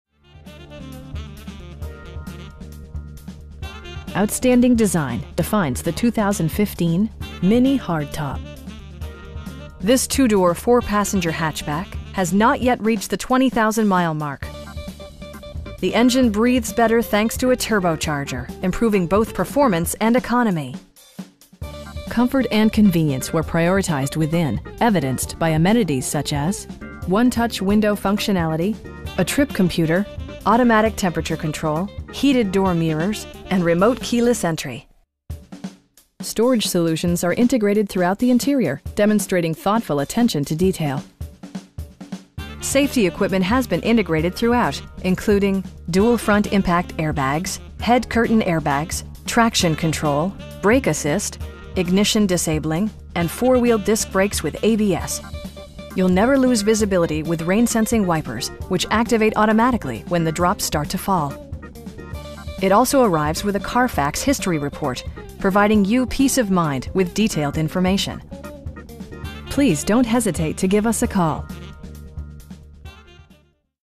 2015 MINI Cooper S Hardtop in Saint Louis, MO 63143
Autohaus BMW
3015 S. Hanley Rd

Outstanding design defines the 2015 MINI Hardtop. This 2 door, 4 passenger hatchback has not yet reached the 20,000 mile mark! The engine breathes better thanks to a turbocharger, improving both performance and economy. Comfort and convenience were prioritized within, evidenced by amenities such as: 1-touch window functionality, a trip computer, automatic temperature control, heated door mirrors, power windows, an overhead console, and remote keyless entry. Storage solutions are integrated throughout the interior, demonstrating thoughtful attention to detail. Safety equipment has been integrated throughout, including: dual front impact airbags, head curtain airbags, traction control, brake assist, ignition disabling, and 4 wheel disc brakes with ABS. You'll never lose visibility with rain sensing wipers, which activate automatically when the drops start to fall. It also arrives with a Carfax history report, providing you peace of mind with detailed information. Our aim is to provide our customers with the best prices and service at all times. Please don't hesitate to give us a call.